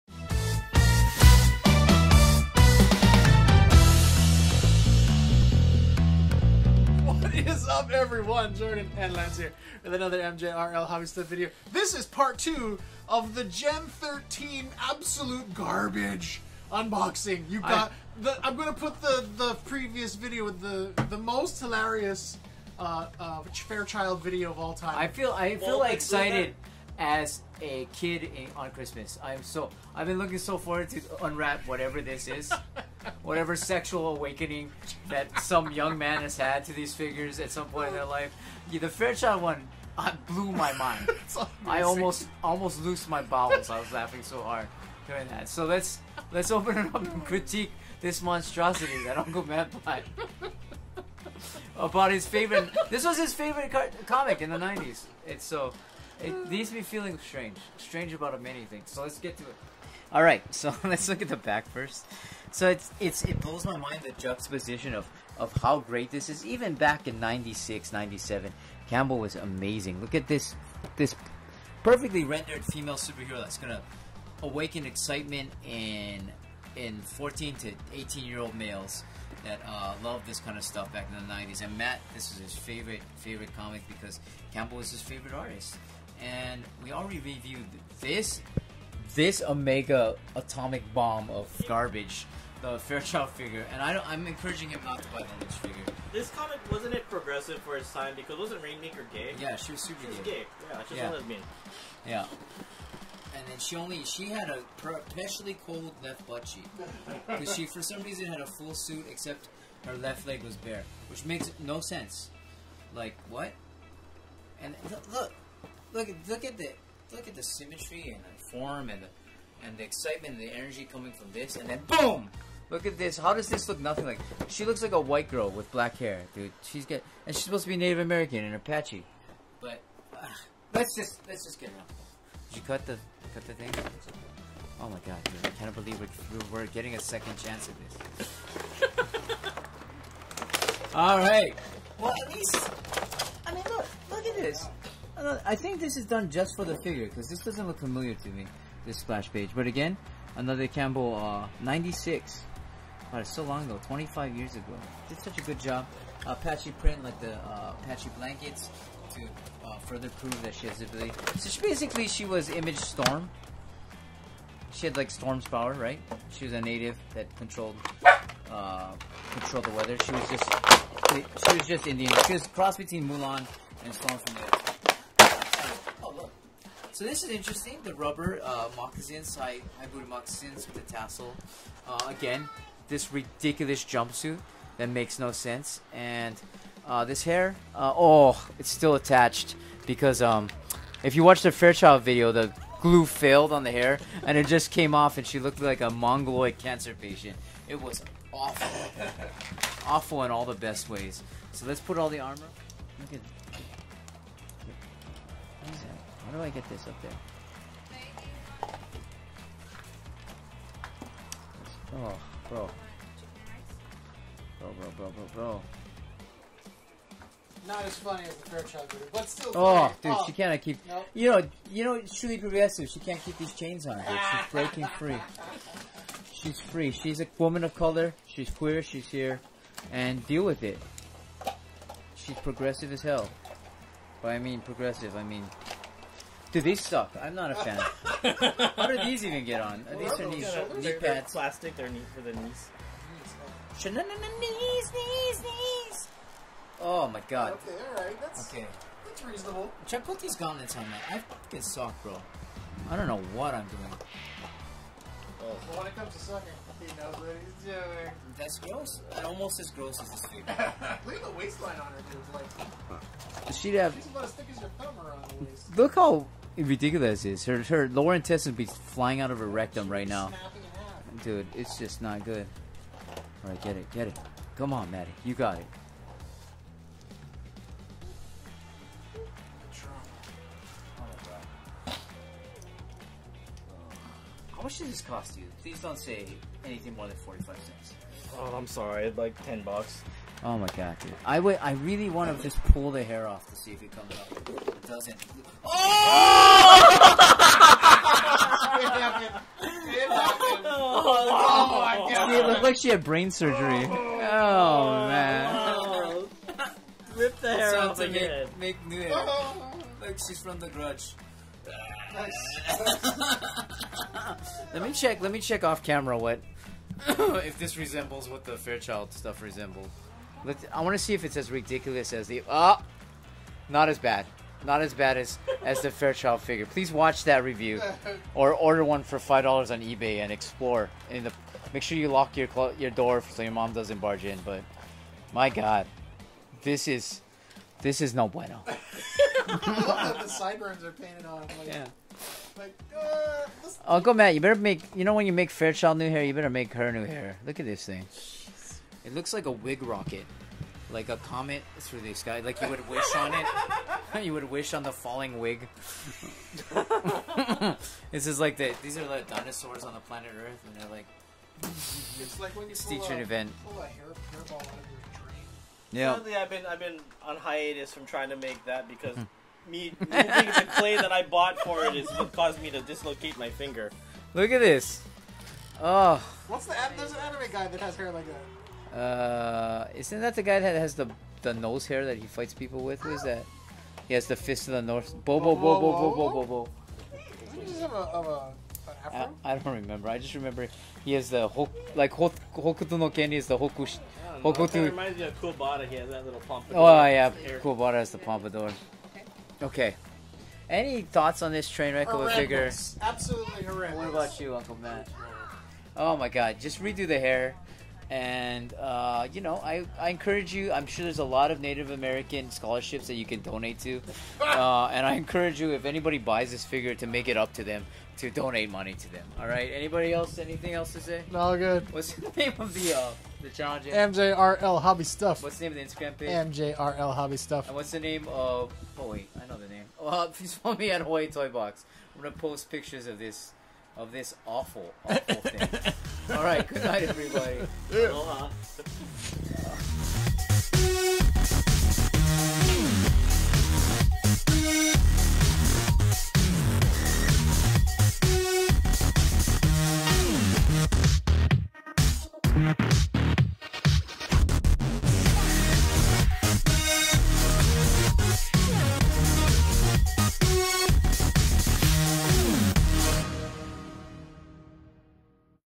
RAINMAKER GEN 13 Unboxing 12 inch Action Figure.
Rainmaker Gen 13 action figure unboxing.

This figure is a 12 inches of crazy, made in the 90’s

Rainmaker was a member of Gen 13 and had powers similar to Storm from X-Men.

Full cloth uniform, and epic hair!

Thank you for the view!